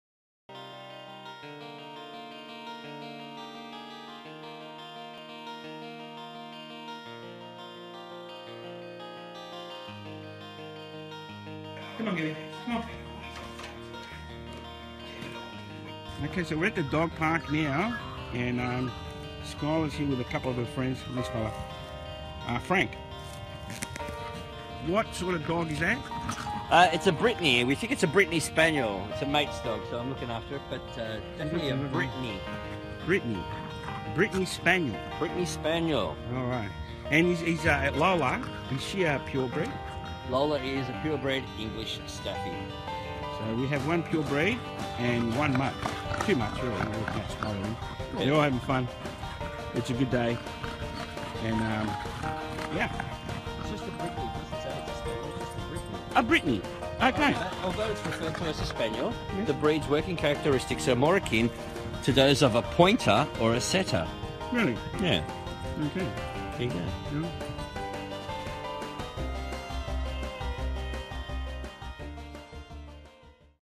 Facebook Page I love cross breeds Upload your doggie Pictures and Videos
Here's a Facebook Page for all doggie lovers, particularly Cross Breeds, as I have always had crossbreeds from growing up with a Labradoodle to my current Girlie "Skyler" which is Border Collie cross Labrador, where you can share your doggie's pictures videos and stories. I'll do my best to keep the page interesting with videos and info from my neck of the woods. Your contribution is welcomed and its good to recognise that Mutts Make Marvelous Mates. see you @ Facebook - I love cross breeds page.